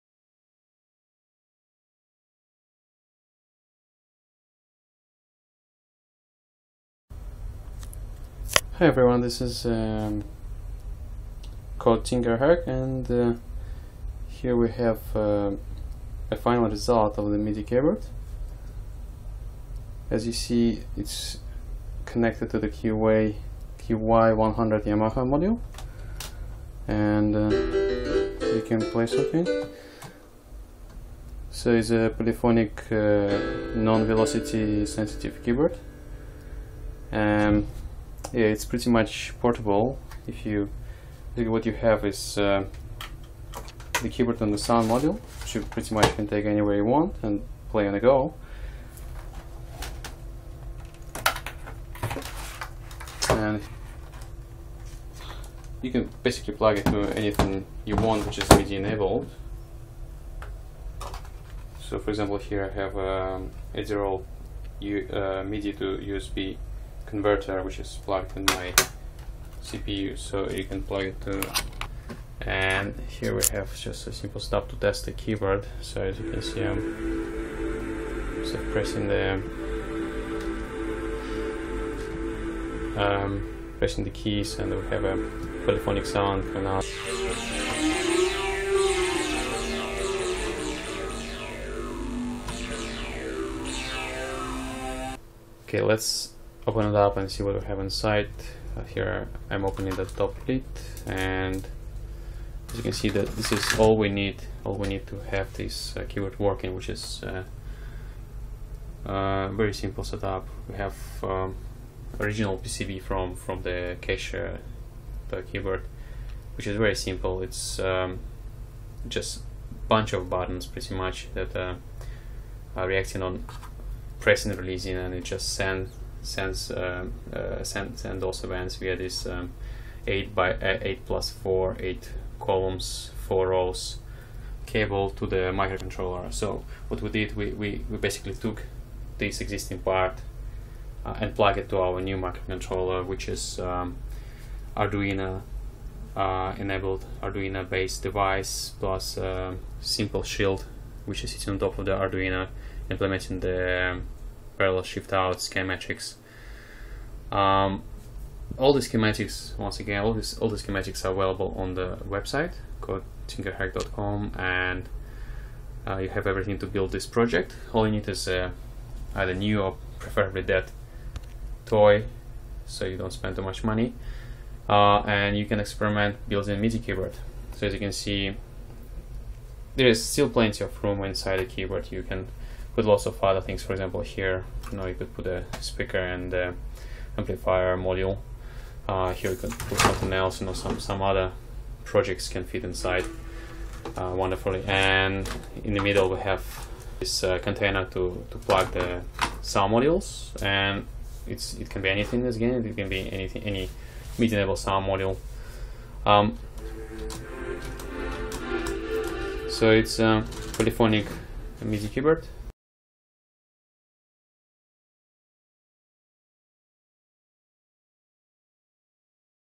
How to turn Piano toy into MIDI keyboard (using Arduino/Atmega)
If you happened to own a broken piano toy and you into Arduino/Atmega you can resurrect it and make a Midi keyboard from it.